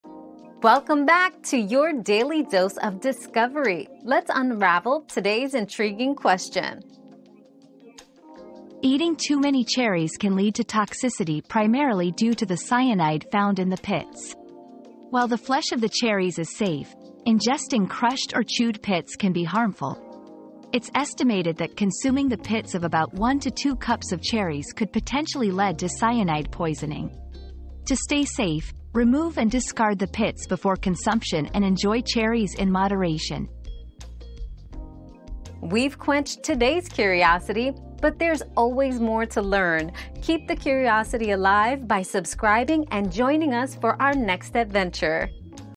How many cherries is toxic?
The Toxicity of Cherries: How Many is Too Many? 👉 Cherry Toxicity Explained 👉 Discover the dangers of eating too many cherries and how it can lead to cyanide poisoning. Learn how many cherries is considered toxic and how to safely enjoy this delicious fruit.

Laura S. Harris (2024, February 6.) How many cherries is toxic?
    🌐 AskAbout.video